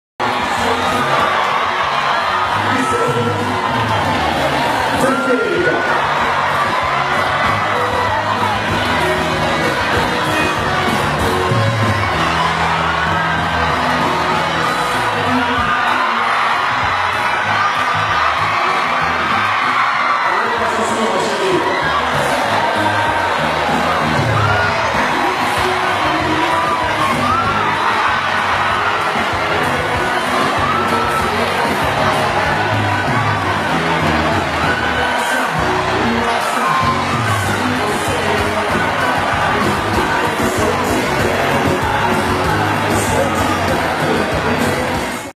Beauty Pageant Fail - Falling in Heels
Poor girl can't walk in the heels! I think I would have practiced a ton before trying to walk on stage in those things! Big heels = big embarassment